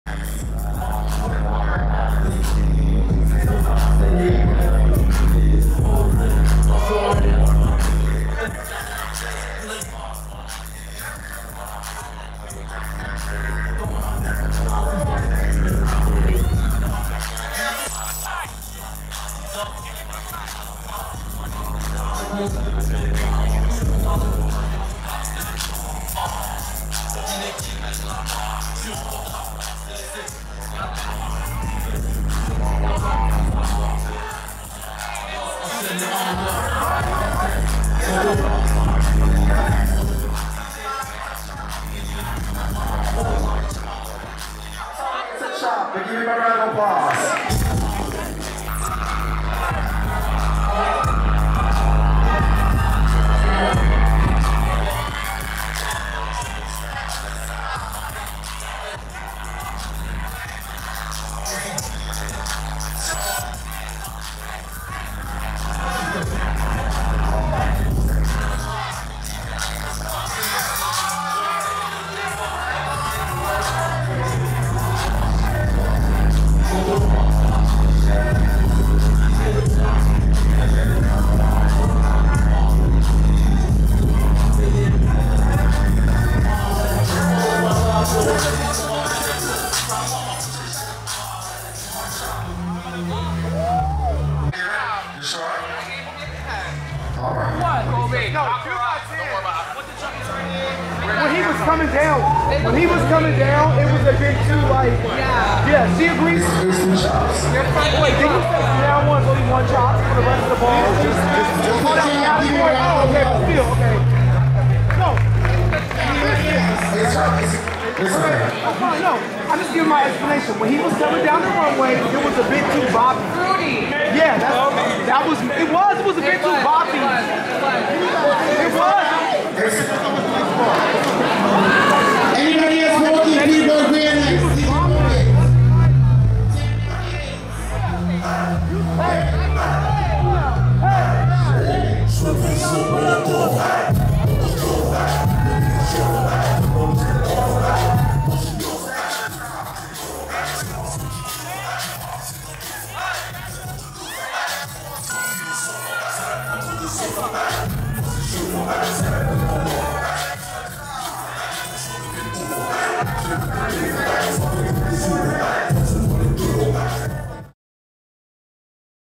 OMG!! POLICE INTERVENTION AT THE EUROPEAN LATEX BALL
Pretty Boy Realness [#1]  - The European Latex Ball by The Legendary Gorgeous Mother Keehdi Gucci.

Paris Ballroom TV :
The Legendary Gorgeous Mother Keehdi Gucci

MC
The Legendary Mother Matyouz Owens